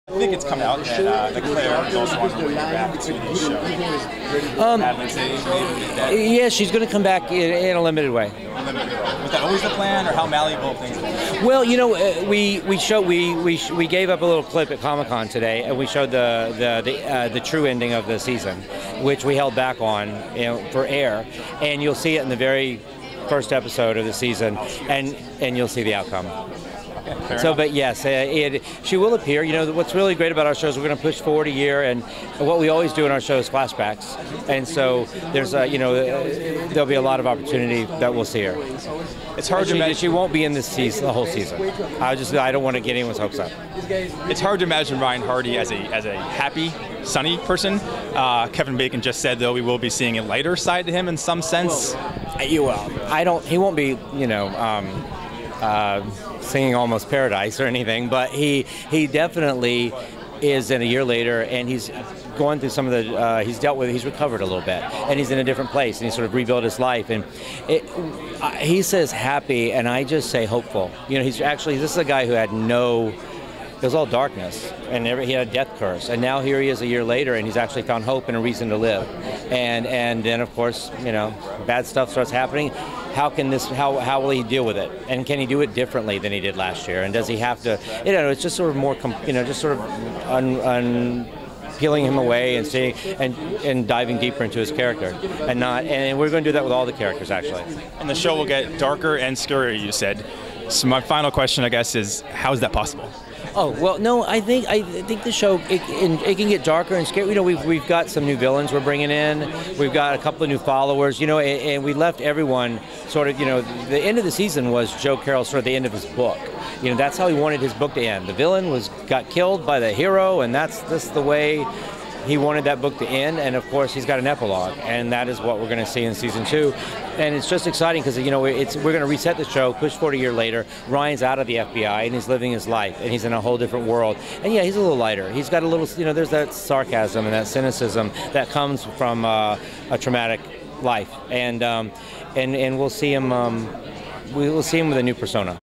Kevin Williamson Speaks on The Following
Kevin Williamson is the man to talk to when it comes to The Following. Watch our exclusive Q&A with the producer now.